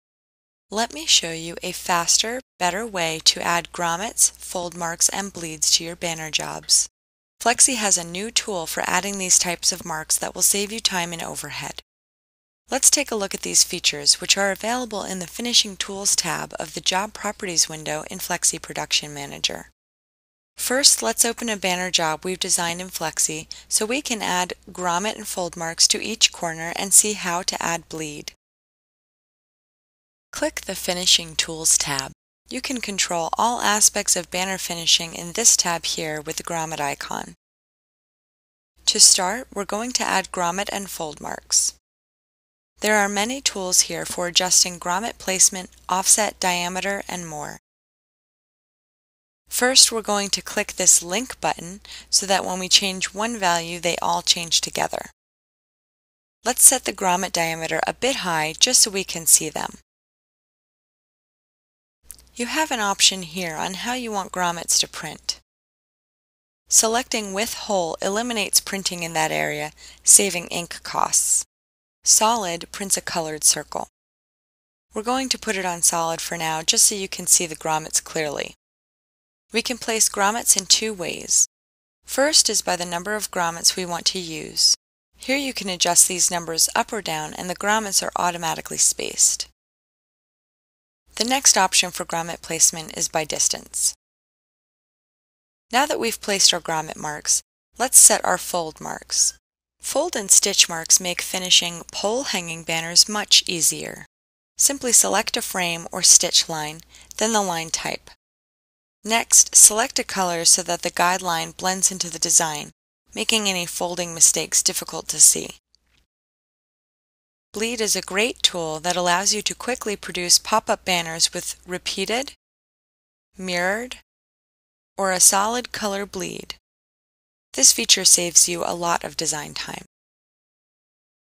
How To Use Flexi's Banner and Canvas Finishing Tools
Learn how to finish banners even faster with Flexi's new finishing tools. Print grommet and fold marks, or quickly add bleed to any job.

This feature is available in FlexiSIGN & PRINT, FlexiSIGN, FlexiPRINT, FlexiDESIGNER and the subscription products Flexi and Flexi Design. Flexi is the most popular wide-format printer RIP and sign making software worldwide.